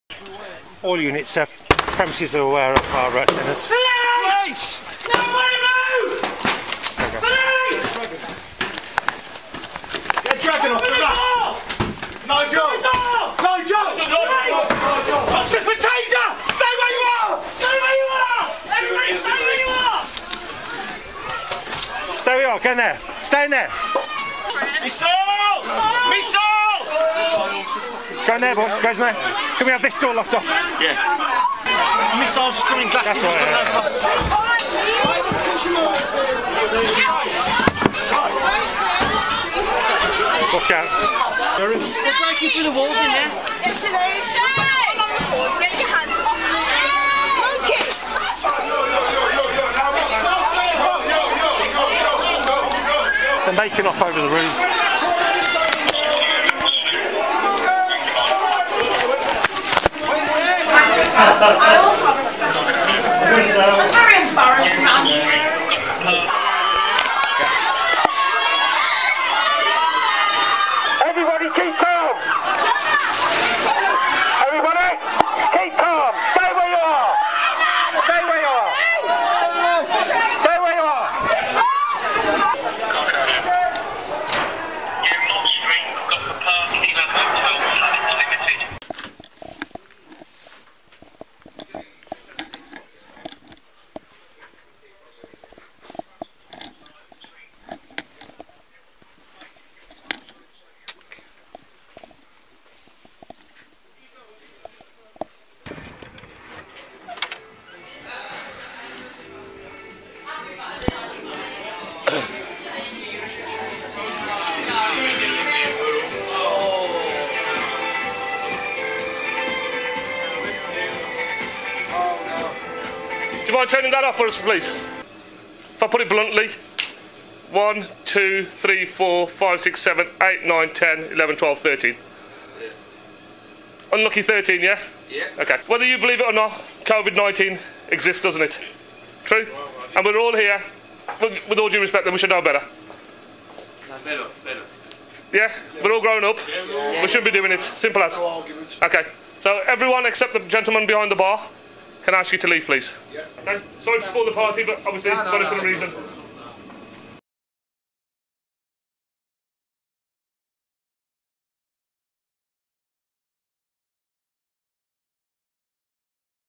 Partygoers flee illegal nightclub & Covid Arms pub in cops bust
Police raided a packed nightclub as dozens of partygoers were caught fleeing via the rooftop on drone footage.

Cops had glass bottles thrown at them as they tried to break up the illegal gathering at a venue near Birmingham’s Jewellery Quarter in the early hours of Sunday morning.

Police had been tipped off that part of a building on Buckingham Street had been turned into a bar, complete with a VIP area and DJ.

After spotting taxis dropping people at the site, officers forced their way in to find around 150 drinkers packed across two floors.

As well as the bar, with neon signs and a dance room, officers came across a recording studio and gym.

Some cops were pelted with bottles and one suffered a minor injury as a door was smashed open by people trying to break out.

Thermal imaging from our drones even captured Covid lockdown flouters trying to flee via the roof.

In total, 50 people are believed to have made a run for it when police arrived.

Some 70 partygoers, however, were issued with £200 fines and the DJ could now face a £10,000 fine.

Continue reading
BASH BUST Shocking moment 150 partygoers flee illegal nightclub with VIP area and DJ after cops caught them using drones